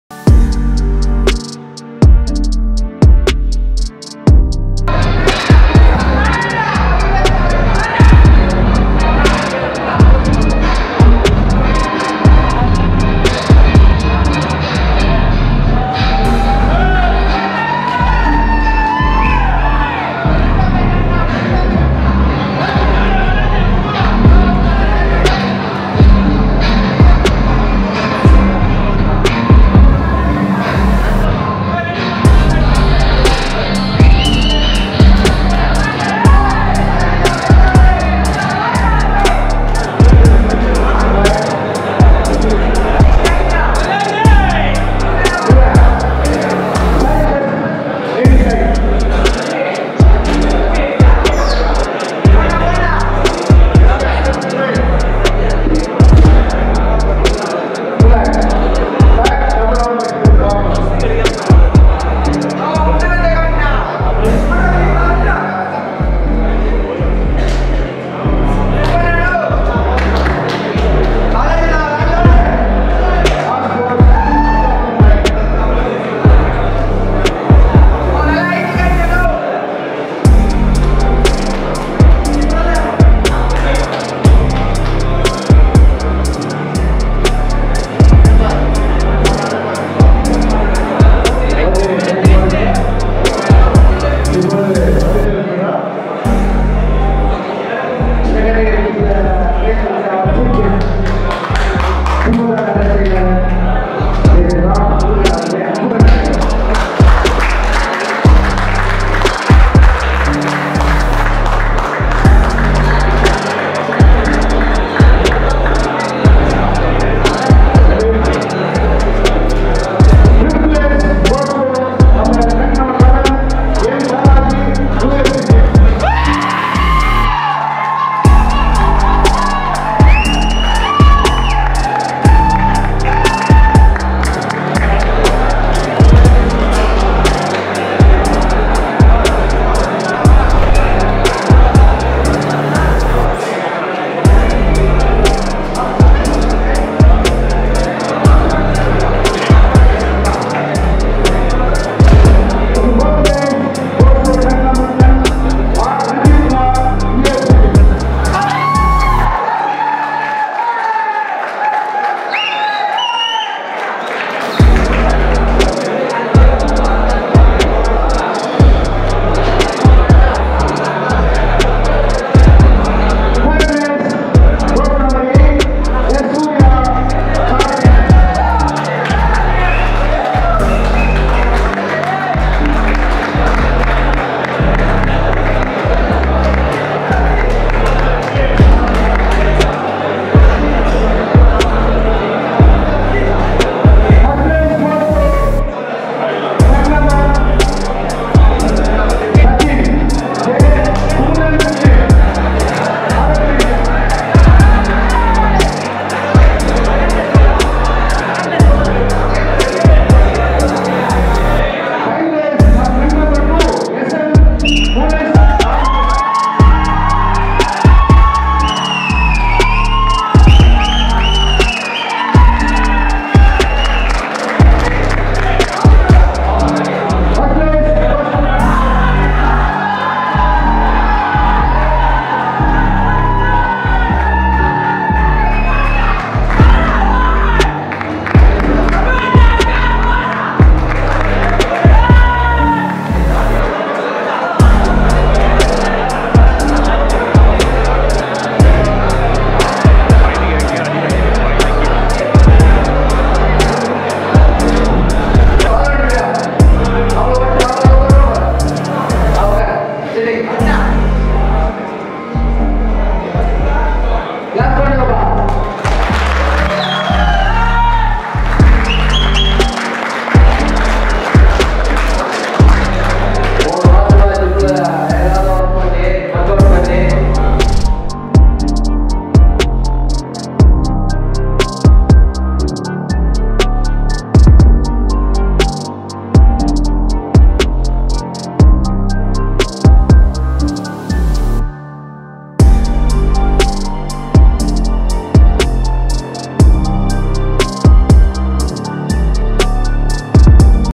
MR.TIRUVANNAMALAI 2021 BODYBUILDING COMPETITION | EPISODE - 11 | Futair Media | TVM Sports
Vj
Arivu

Cinematographer
Ranga Rajan Kumar

Editor:
Karthik Velavan

Producttion manager:
Ranga Rajan Kumar

CEO
santhoshkumar

Follow us on other social media's